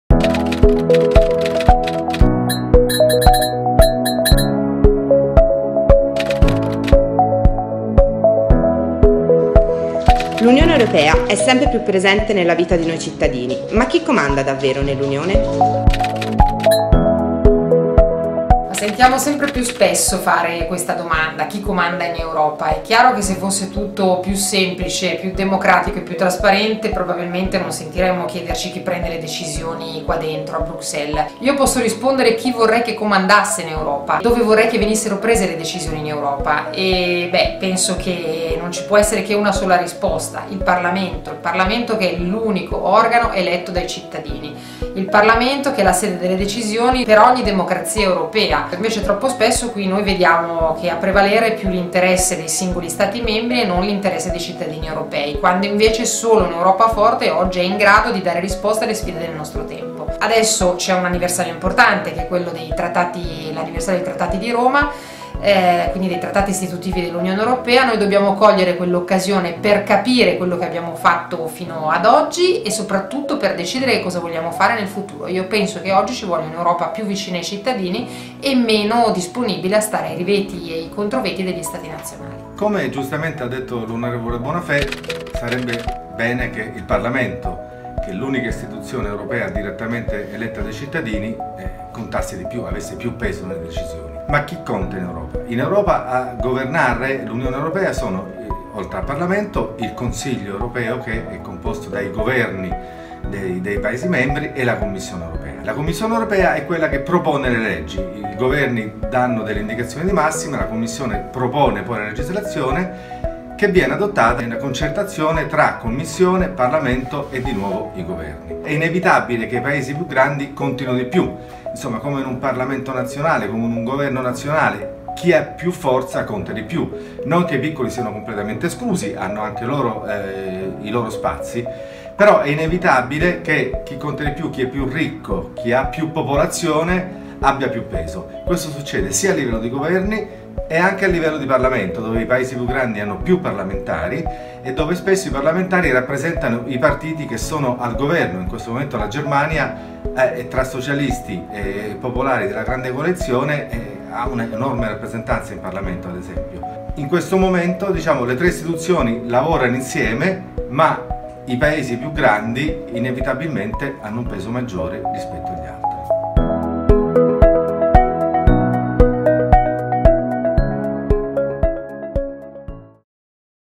C'è posta per l'ue - Chi comanda in Europa?
Un progetto di Eunews e Tiscali che porterà i lettori a contatto diretto con Bruxelles. Per saperne di più e inviare le tue domande